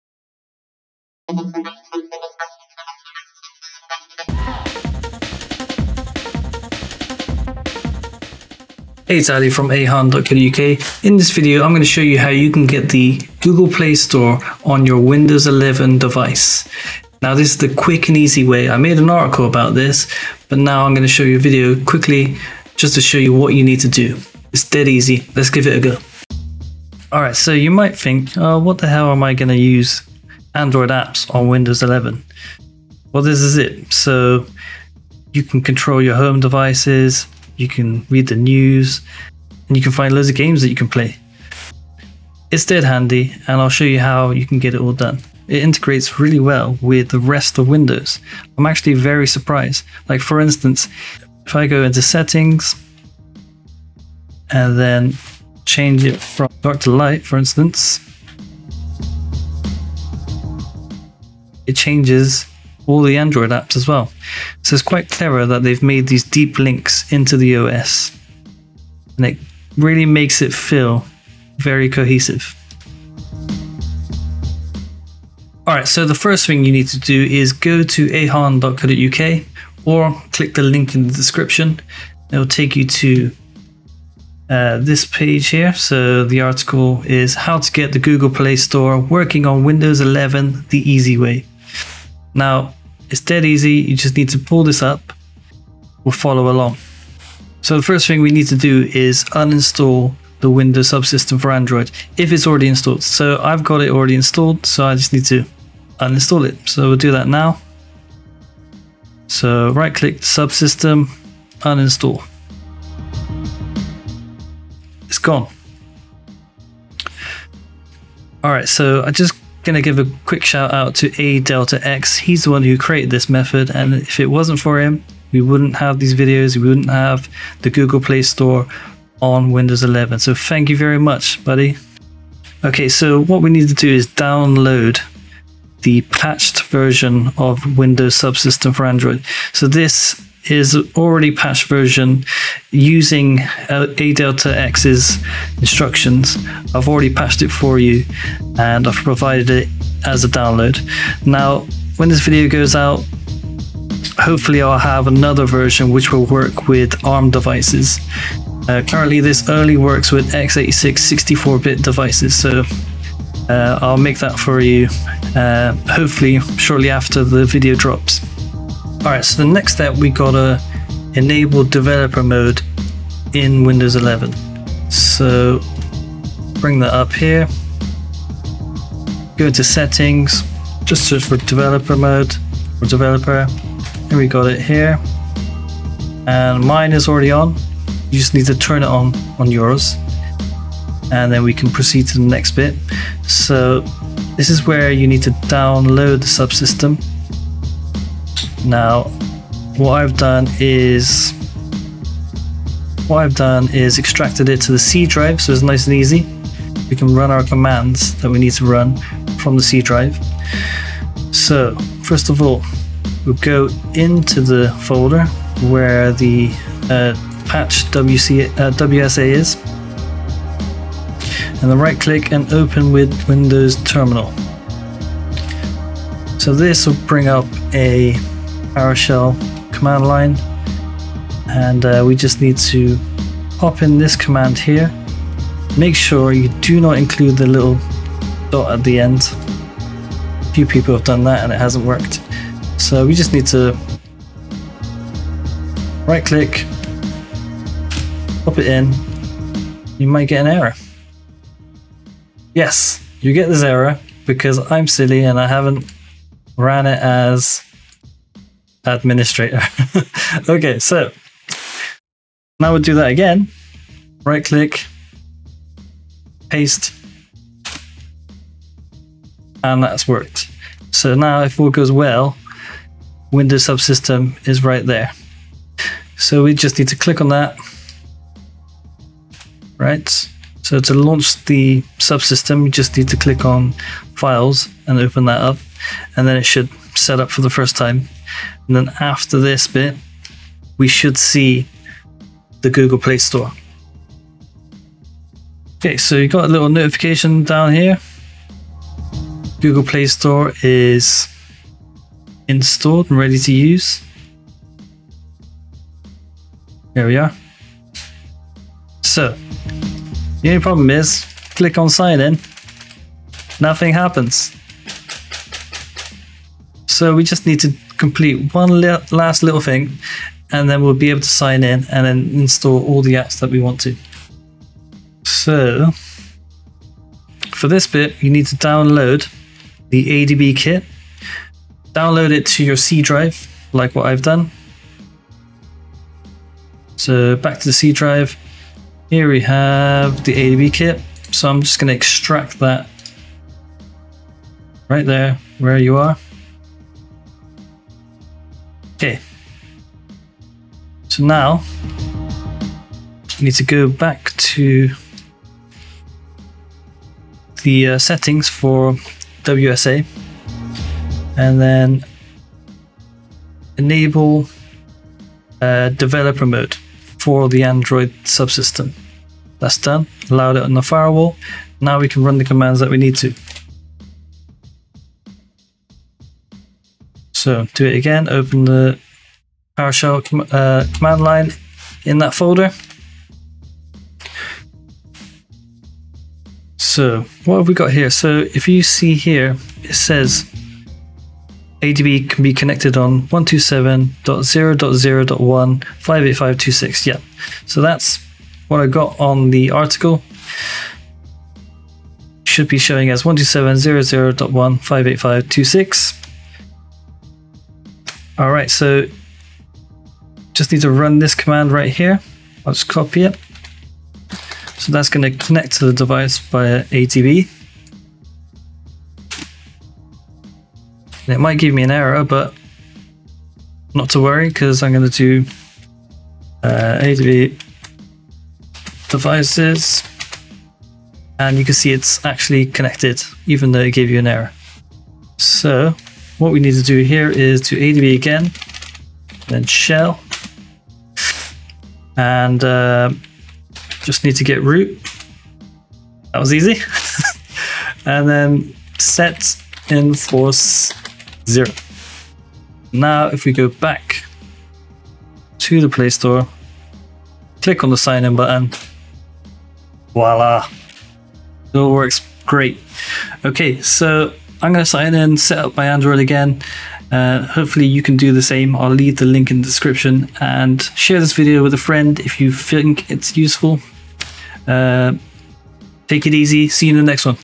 The Easy Way to Get The Google Play Store Working on Windows 11 🕒 less than 5 minutes! Android 12.1
Get the Google Play Store on Windows 11 in less than 5 minutes! 🕒 Until today, it was impossible to launch the Google Play Store and sign in to your Google account successfully. This video shows you how to install Google Play services and sign in to the Google Play Store on Windows11.

Article updated with Android 12.1 download links!

Windows Subsystem for Android™️ enables your Windows 11 device to run Android applications available in the Amazon App store.

By enabling the Windows Subsystem for Android in Windows 11, you're enabling your Windows device to run Android apps. Android applications are downloaded from the Amazon Appstore on your Android device.

Microsoft released the Windows Subsystem for Android to Windows Insiders on the Beta build of Windows 11 earlier this week.

It didn't take long for developers to tinker with the build of Android.

00:00 Intro
00:26 Google Play Services Apps working showcase
00:46 Windows 11 Android Integration
01:25 HOW-TO intro
01:53 Uninstall unpatched Windows Subsystem for Android
02:40 Download patched Windows Subsystem for Android
03:00 Enable Developer Options in Windows 11 Settings
03:40 Install WSA with PowerShell script
04:38 Enable Developer Mode in the Windows Subsystem for Android
04:51 Start WSA
05:06 Launch the Google Play Store
05:26 Download ADBKit for WSA
06:06 Allow ADB access through the Windows Firewall
06:51 ADB connect to Windows Subsystem for Android
07:21 Root Windows Subsystem for Android
07:31 Enable Google Play Services Sign in for Windows Subsystem for Android
07:40 Outro